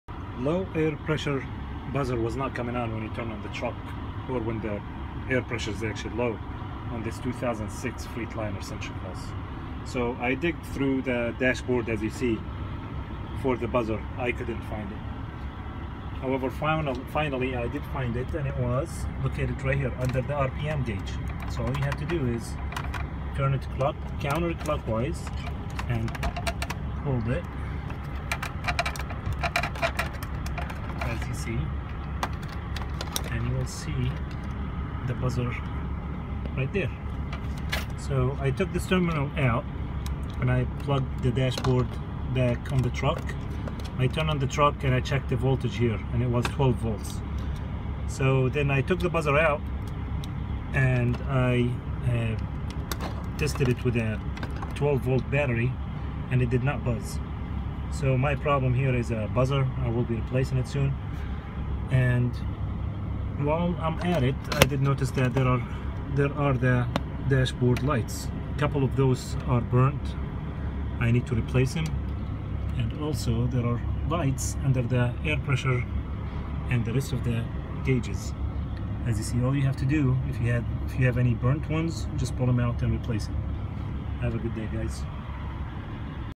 Low air pressure buzzard location and troubleshooting 2006 freightliner century class
Low air pressure buzzer replacement 2006 freightliner century